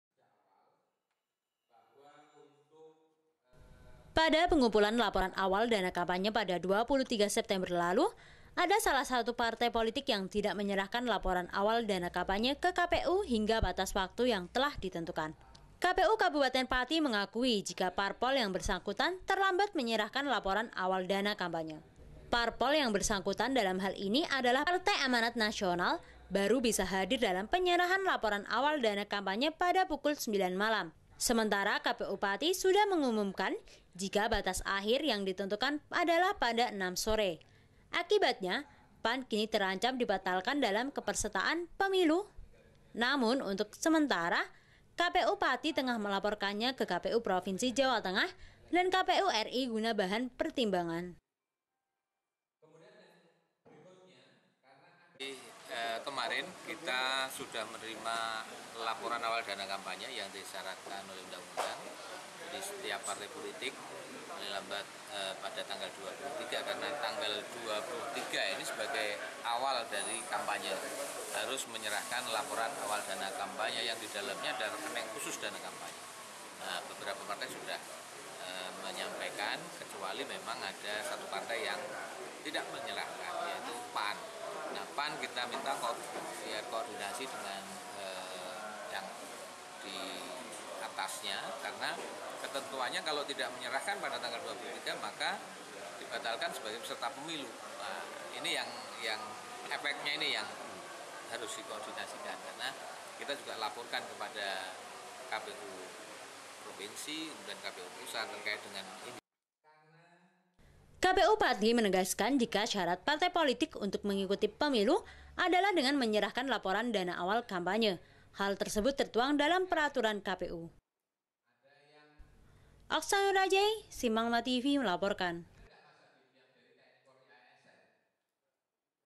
PAN Terancam Batal Ikut Pemilu
Kpu Kabupaten Pati Belum Menerima Laporan Awal Dana Kampanye Partai Amanat Nasional. Kini Pan Terancam Dibatalkan Dari Kepesertaan Pemilu 2019. Kpu Pati Menyebutkan Jika Syarat Parpol Bisa Menjadi Peserta Pemilu Adalah Dengan Menyerahkan Laporan Awal Dana Kampanye.